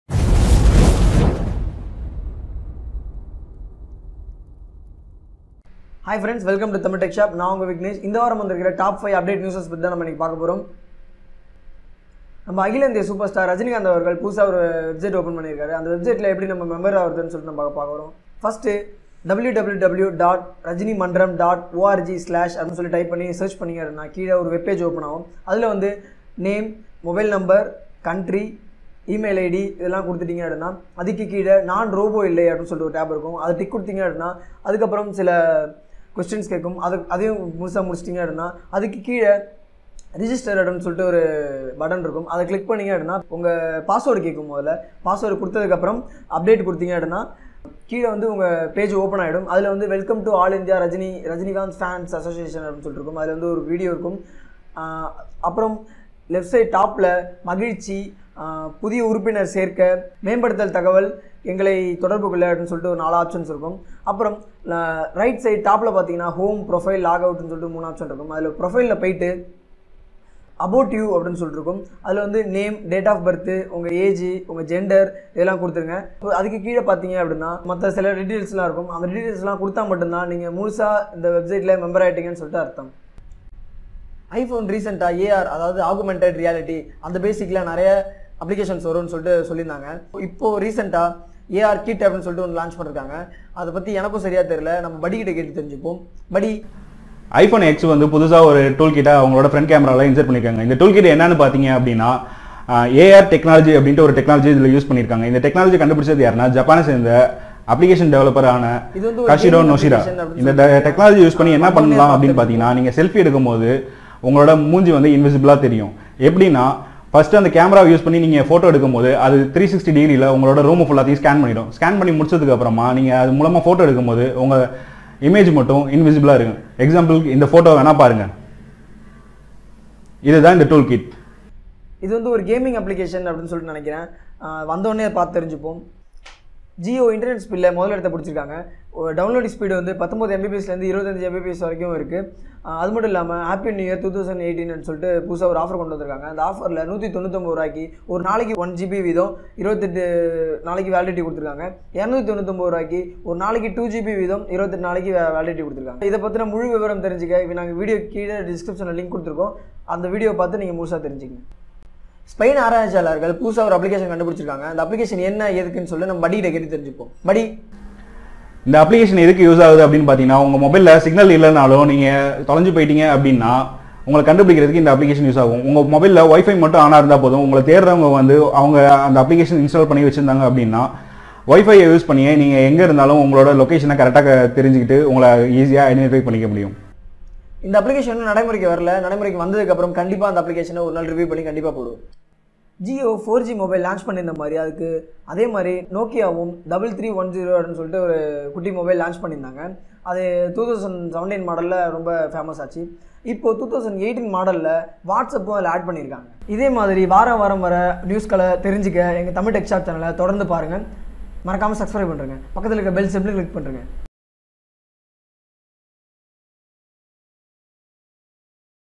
TAMIL TECH BUDDY #1 RAJNIKANTH WEBSITE,I PHONE X (AR),JIO LEADS NET SPEED,NOKIA 3310,etc
1 : ரஜினி அவர்கள் துவங்கியுள்ள ரசிகர்மன்ற வெப்சைட்டில் நாம் எப்படி மெம்பெர் ஆவது என்றி குரியுளோம்

2 : I PHONE X போனில் உள்ள  AR(AGUMENTED REALITY) அமைப்பை பற்றி குரியுளோம்

3: JIO நிறுவனம் இன்டர்நெட் வேகத்தில் முதலிடம் பிடித்துள்ளது என்பதையும் மற்றும் JIO கொண்டுவந்துள்ளHAPPY NEW YEAR 2018 OFFER பற்றியும் குரியுளோம்

4 : ஸ்பெயின் நாட்டு ஆராய்ச்சியாளர்கள் கண்டு பிடிக்கப்பட்டுள்ள ஒரு அப்ளிகேஷனை பற்றியும் குரியுளோம்

5 : NOKIA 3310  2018 மாடலை பற்றியும் குரியுளோம்